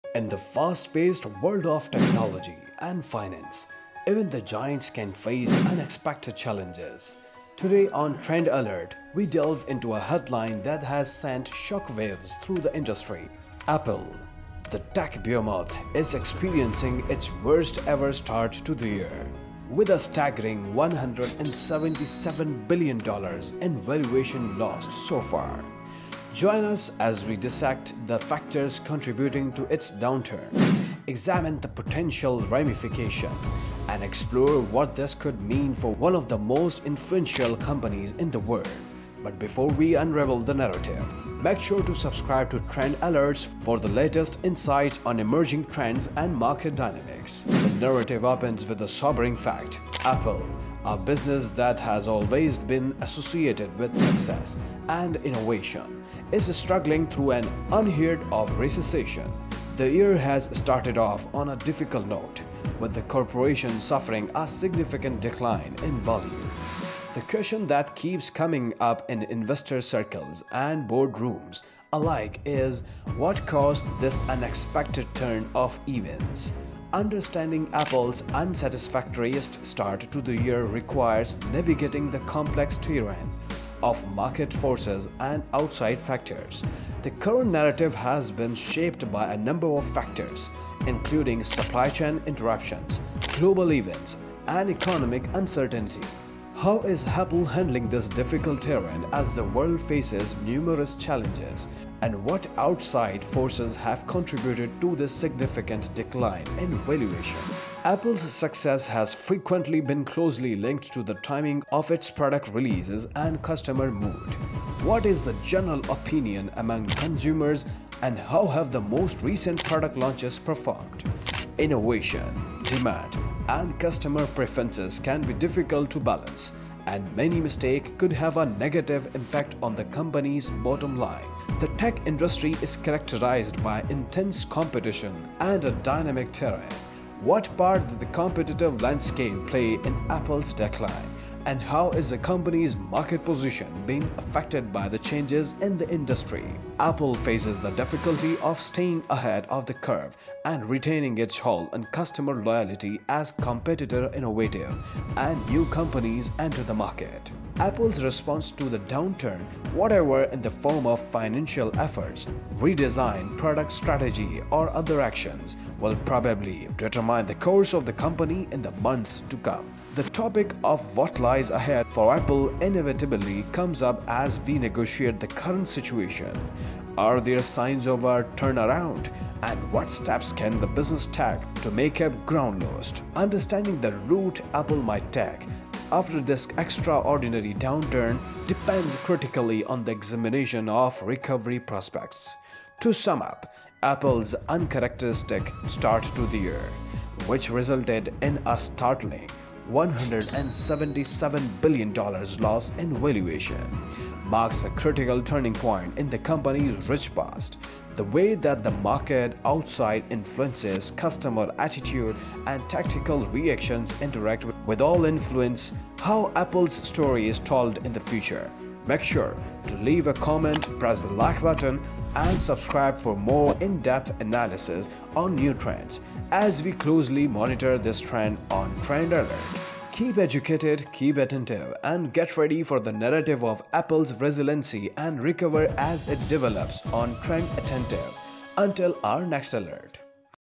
Apple CRASHES: $177 Billion Valuation Loss
Apple CRASHES with a jaw-dropping $177 billion valuation loss, marking its worst-ever start to the year. Join us on Trend Alert as we dissect the critical factors behind this financial upheaval.

💻 How is Apple navigating these turbulent waters? Examine the timing of product releases, customer sentiments, and the fierce competition in the tech industry that may have contributed to this unprecedented decline. What actions will Apple take to counter this downturn, be it financial strategies, product redesigns, or other decisive moves?